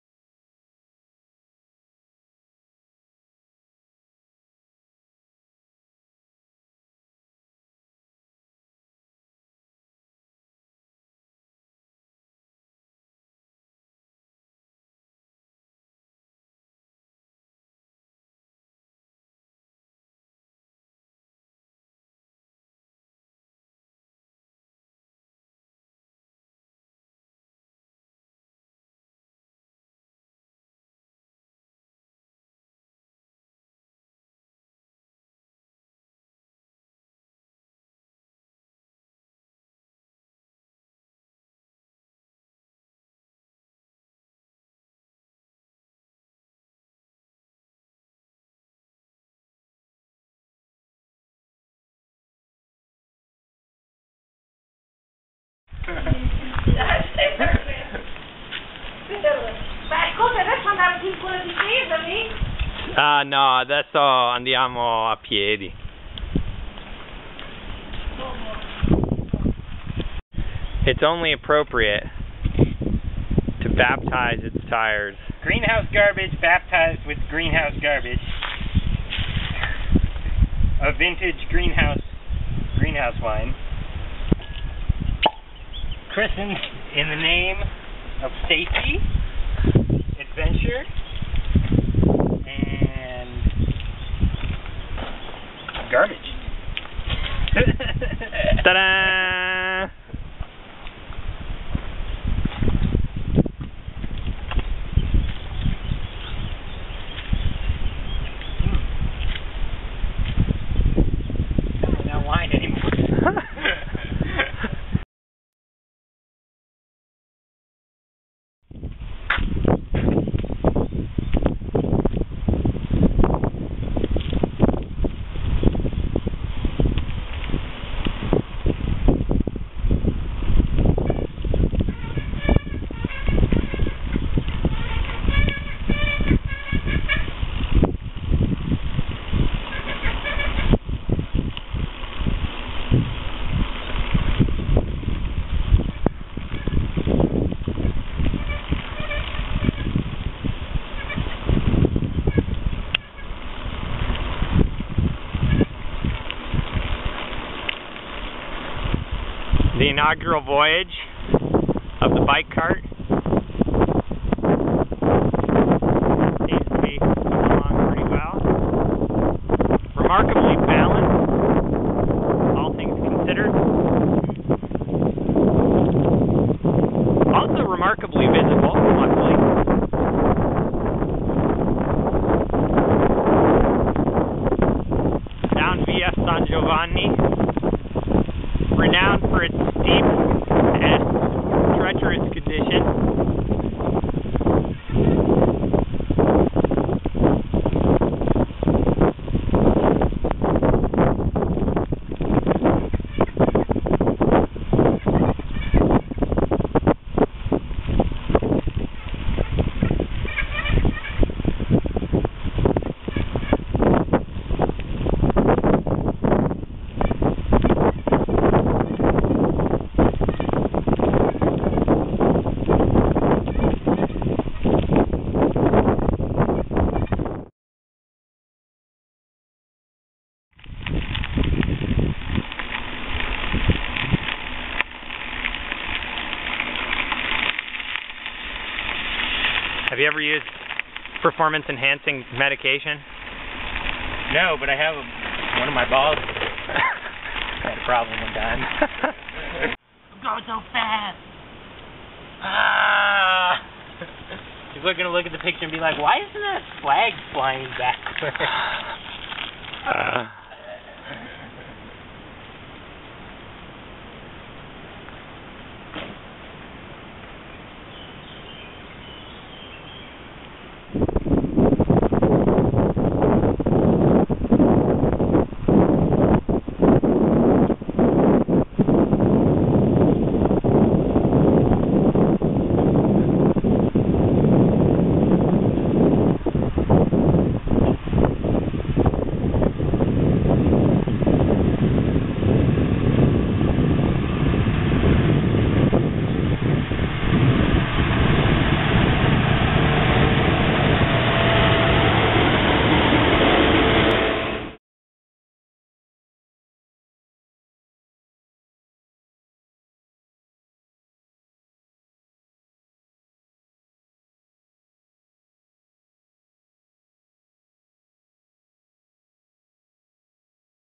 Shopping cart bike trailer- Italy to Turkey
We built this bike trailer out of a shopping cart, bike parts, a satellite dish, and various things we emptied out of an old greenhouse (including parts of the greenhouse itself.) We bolted or clamped all the joints without welding.  The goal is to reach Istanbul, Turkey from Forli, Italy.